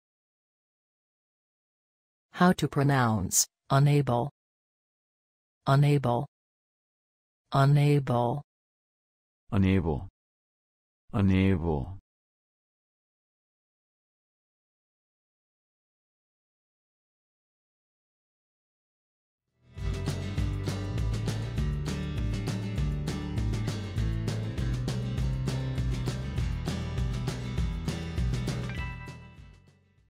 How to say Unable
How to pronounce Unable in English? Listen with us.
What is the correct pronunciation of the word Unable in everyday English? Speak as the Americans.
How to write the word Unable? Watch and learn.

Learn how to pronounce Unable.